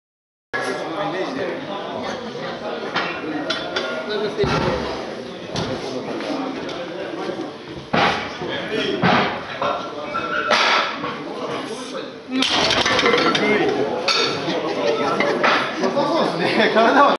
Yuki Fukushima (JPN) warm up 4 - 2015 IPF Men's World Benchpress Championships
IPF World Benchpress Championships 2015
in Sundsvall/Sweden
class: Men's open -83 kg